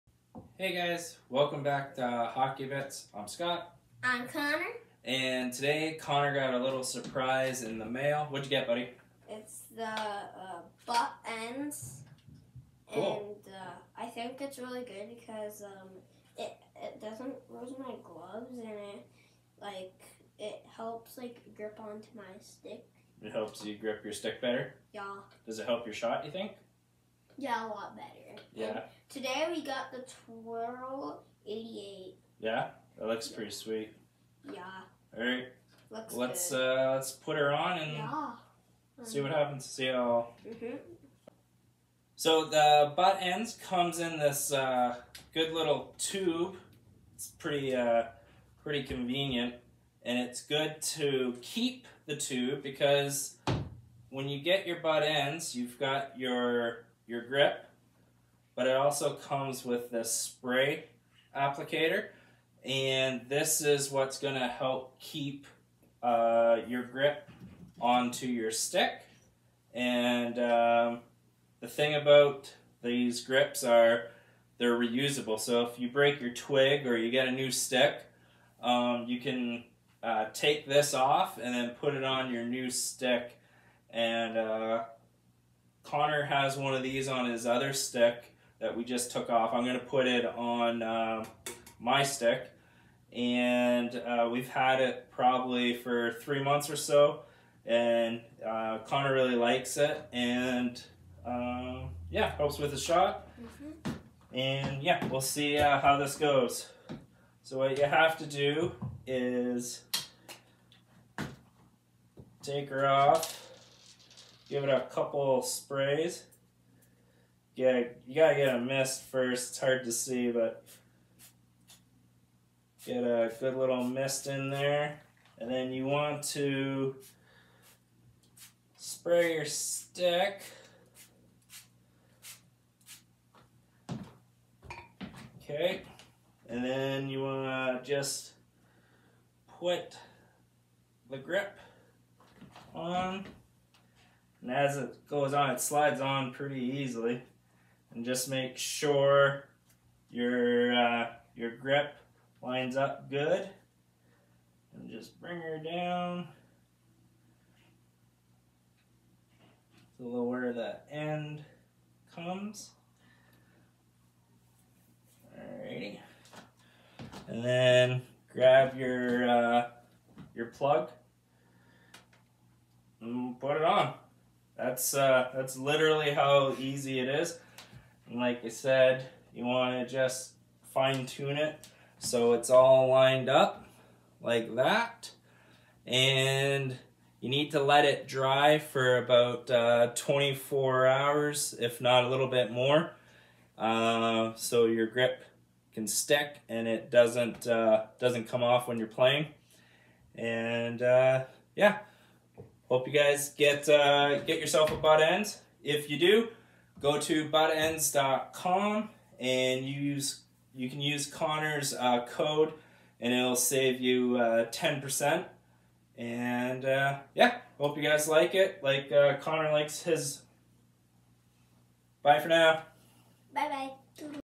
ButtEndz Unboxing and Application
Connor got a new ButtEndz today, and he was stoked! We did a video to show you how easy it is to put one on. Check out his Instagram account for frequent hockey videos: @hockey_bit